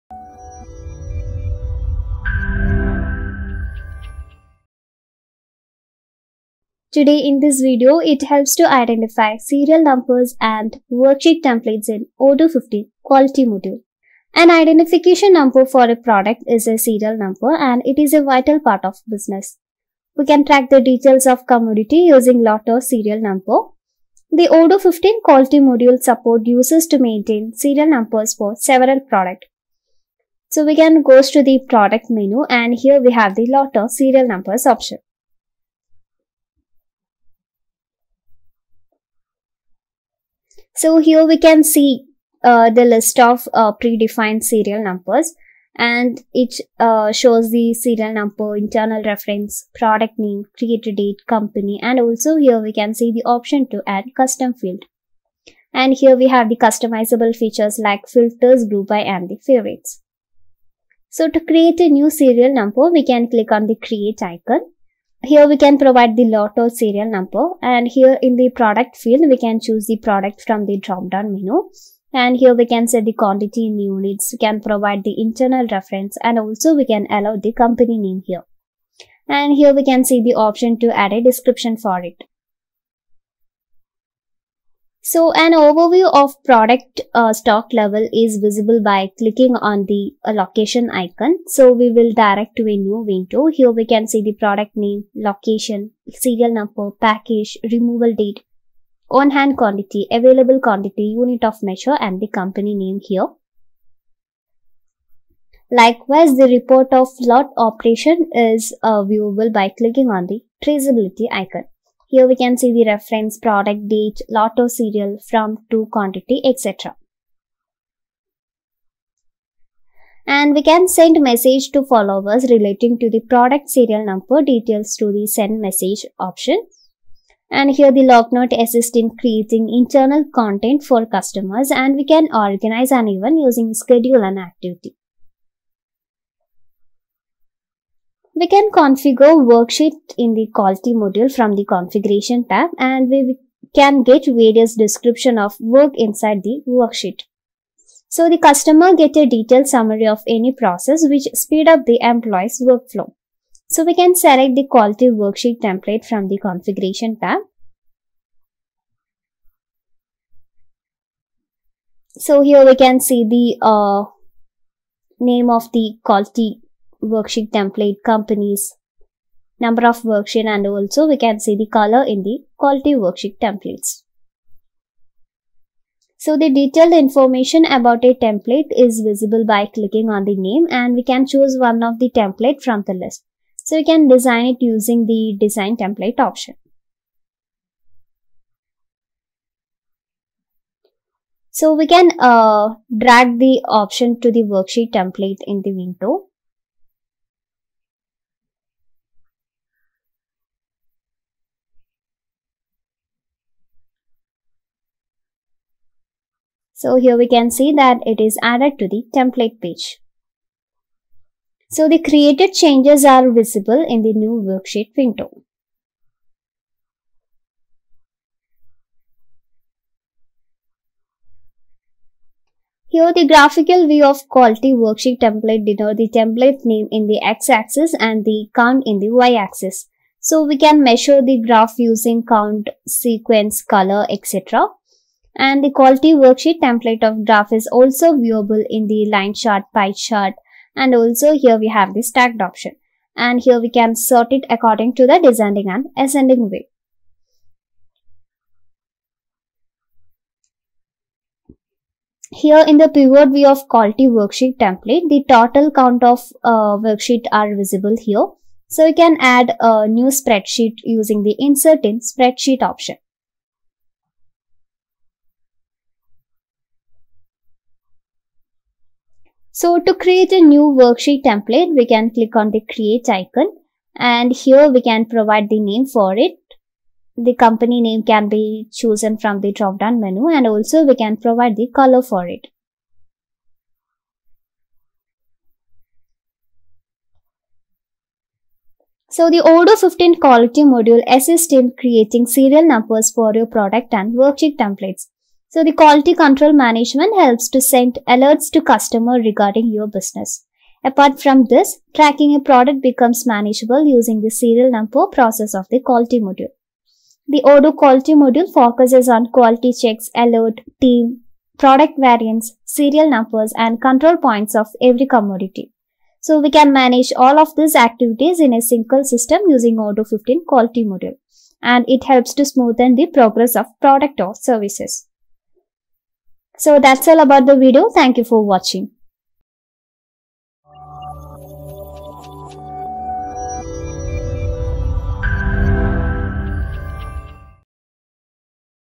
Serial Numbers & Worksheet Templates in Odoo 15 Quality Module | Odoo 15 Enterprise Edition
This video helps you identify Serial Numbers and Worksheet Templates in Odoo 15 Quality Module. The companies get exceptional stock control using the serial numbers of products.

Video Contents
00:00   Introduction
00:33   Lot/Serial Number
01:00   Create New Serial Number
02:37   Quality Worksheet Templates

Serial numbers arrange in numerical/alphabetical orders, different from other bar codes. Odoo Quality module assists you in categorizing the products based on serial numbers.

The primary advantage of a serial number is tracking the background details and raising stock accuracy. On the other side, you can create a Worksheet Template relating to products in the Odoo Quality module.

We can also figure out the teams, Quality checks, product variants, Serial Numbers,  Alerts, and Control Points through the Odoo 15 Quality module.